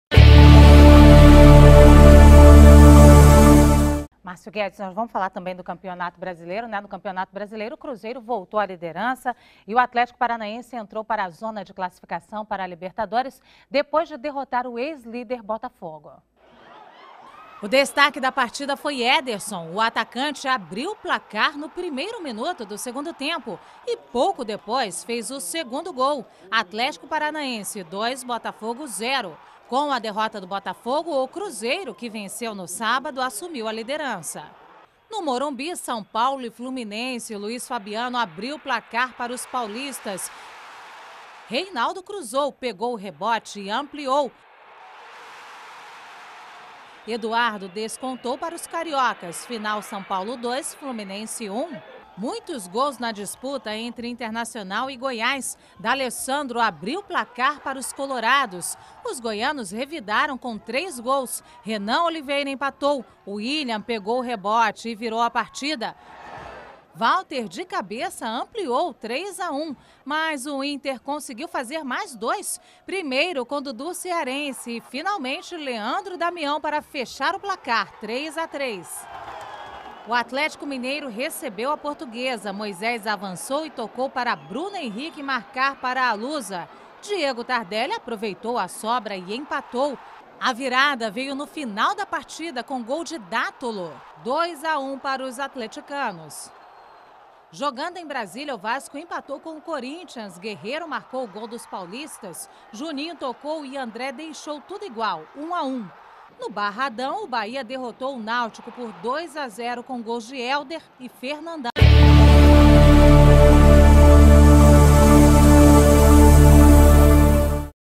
Gols da rodada - Cruzeiro volta à liderança do Brasileirão
São Paulo volta a vencer. Vasco e Corínthians empatam em Brasília em jogo com briga entre torcedores.